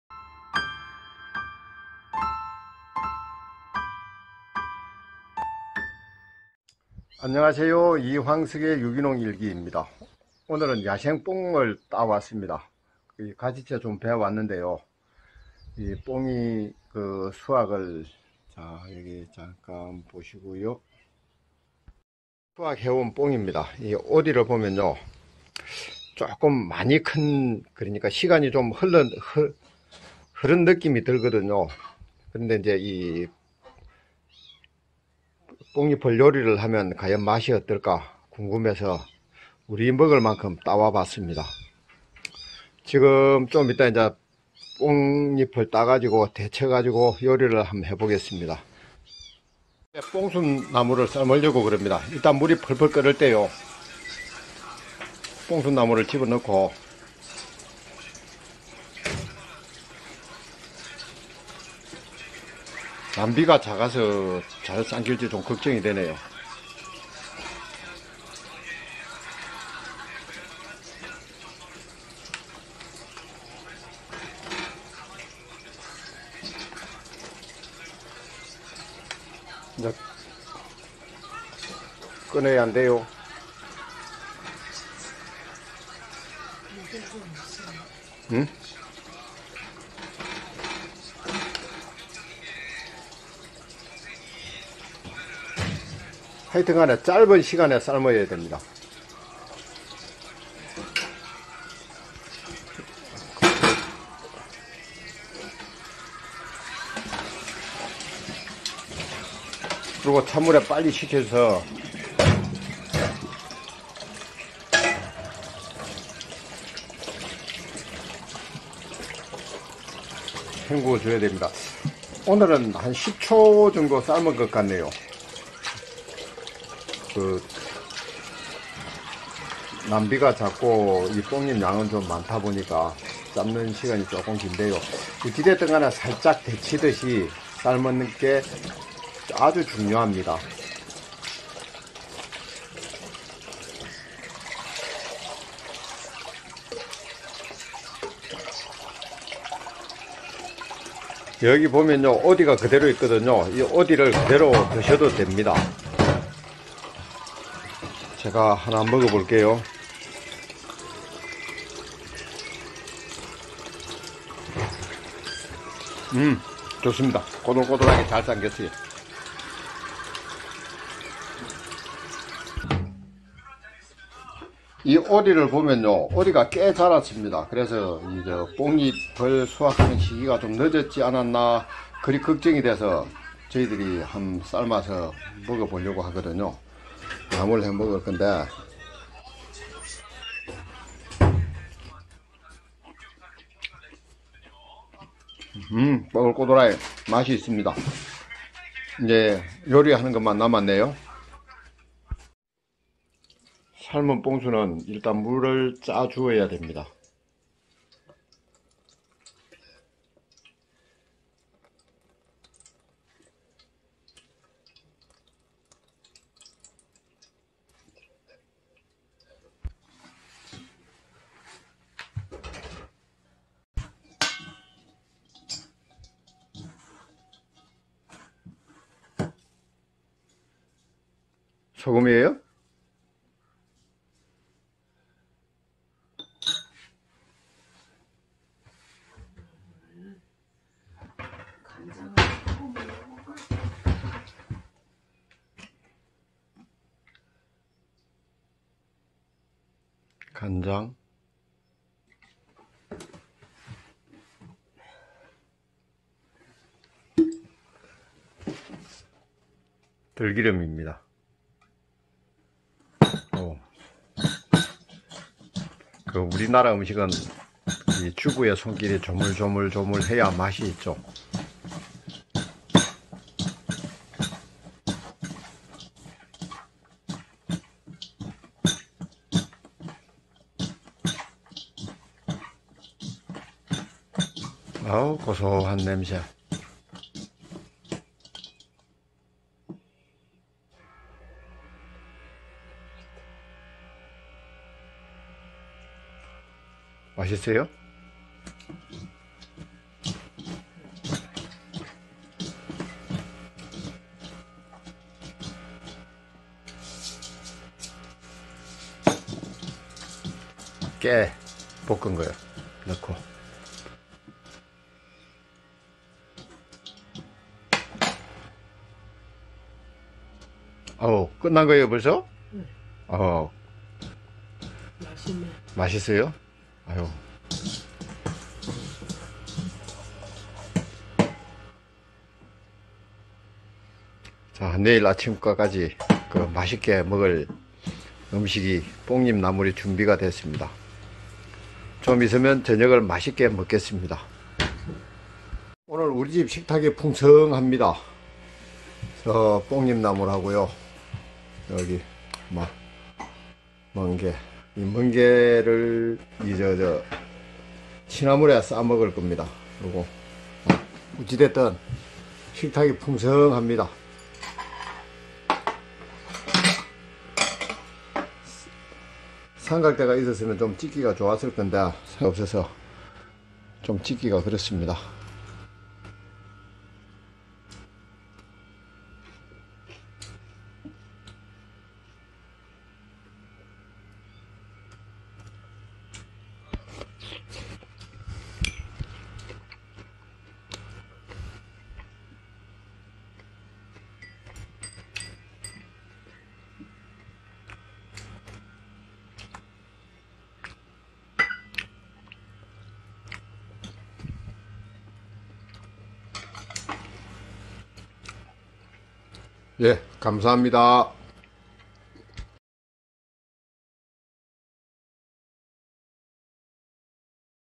뽕잎 요리
뽕순을 따서 요라를 하는 과정입니다,
뽕이 요리는 뽕순을 데치는 것이 제일 중요 합니다.
뽕순을 데칠때 님빙의 커기와 봉순의 양에 따라 데치는 시간이 달라지갰지만 10초 이내에  데쳐야 됩니다.